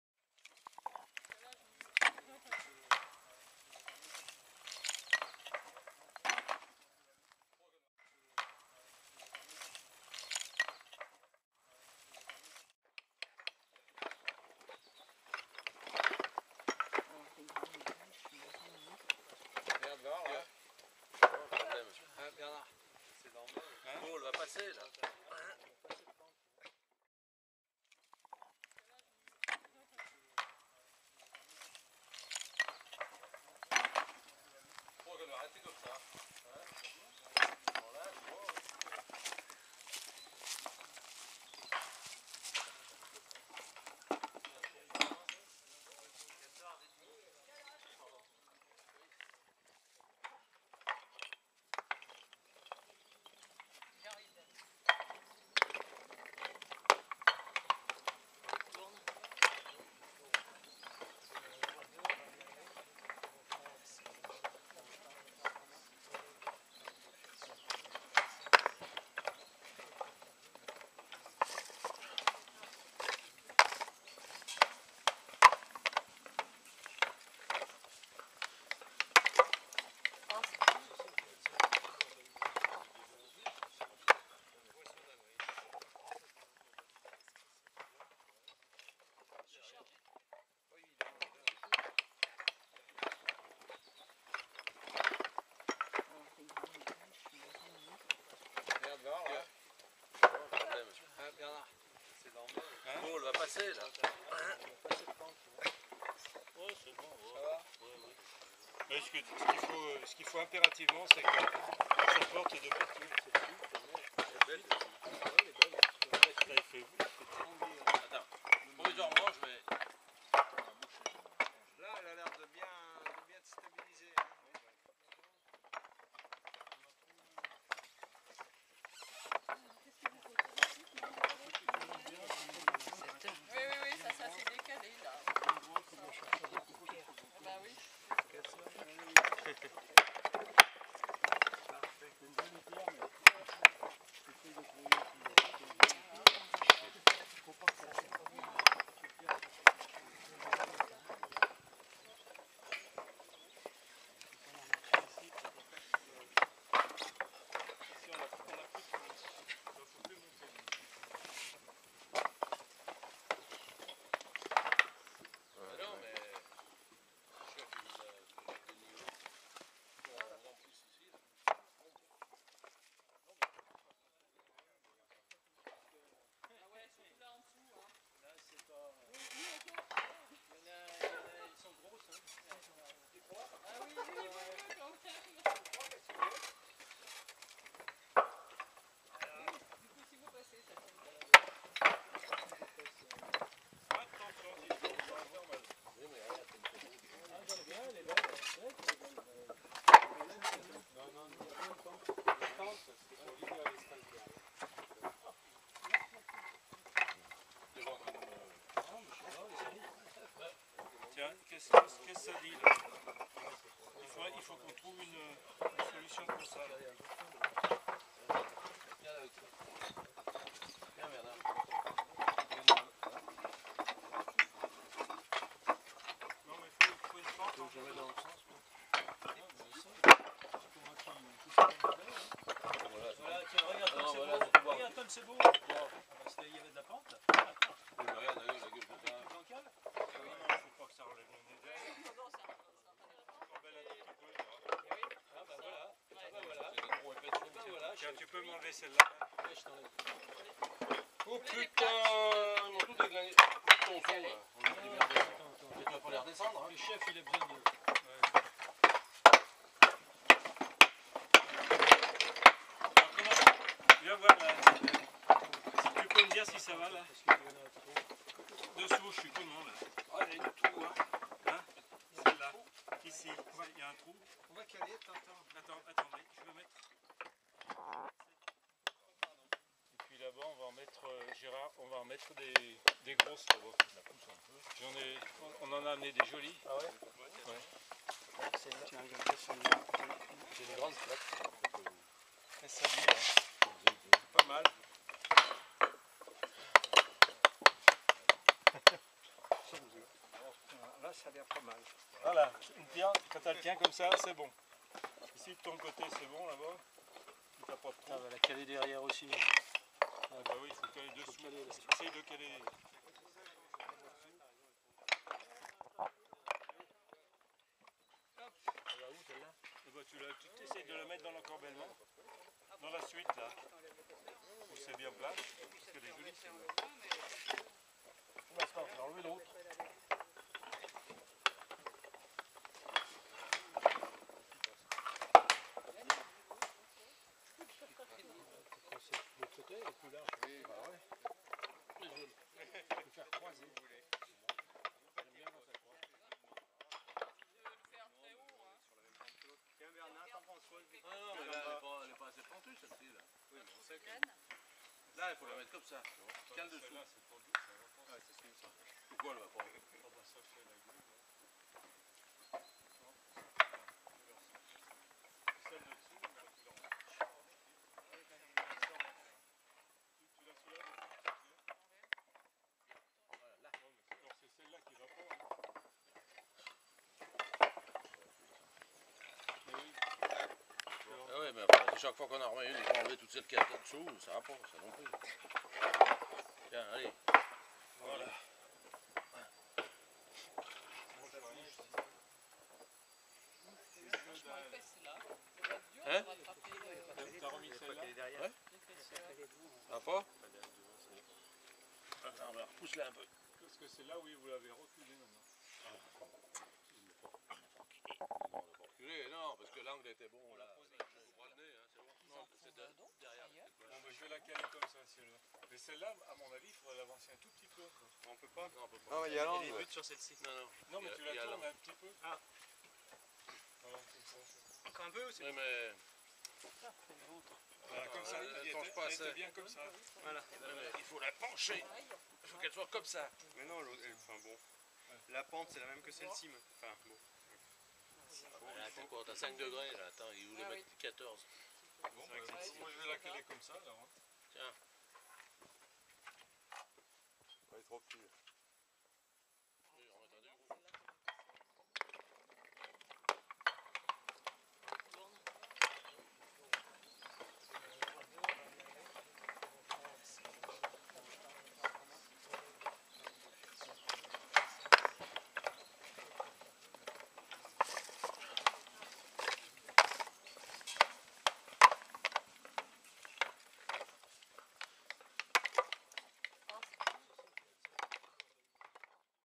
Chantier pierres sèches - Encorbellement Caborne Berger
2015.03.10 Saint Cyr au Mont d'Or (69)
Encorbellement de la caborne Berger par les bénévoles de l'Association Cabornes et Petit Patrimoine du Mont d'Or, qui contribue à la restauration du patrimoine bâti en pierres sèches.